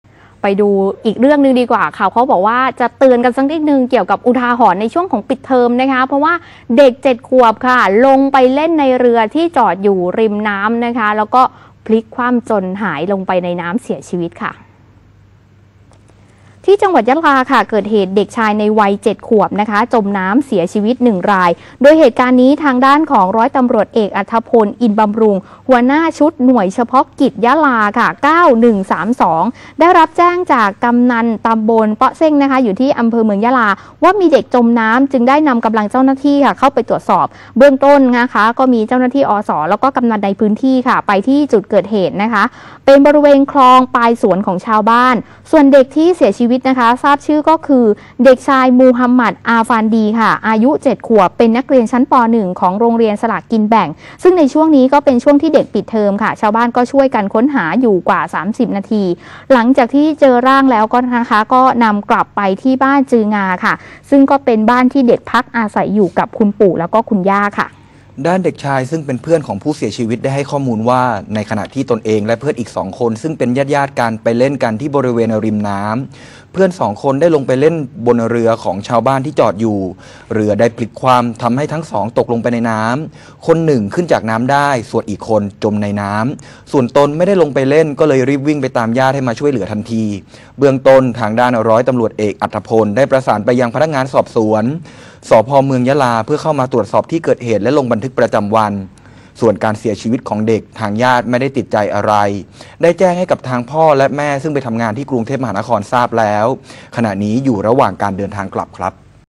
อุทาหรณ์ช่วงปิดเทอม เด็ก 7 ขวบ จมน้ำเสียชีวิต
ข่าวเด่น ประเด็นใต้ ประจำวันพฤหัสบดีที่ 21 มีนาคม 2567 -ยะลา - อุทาหรณ์ช่วงปิดเทอม เด็ก 7 ขวบ จมน้ำเสียชีวิต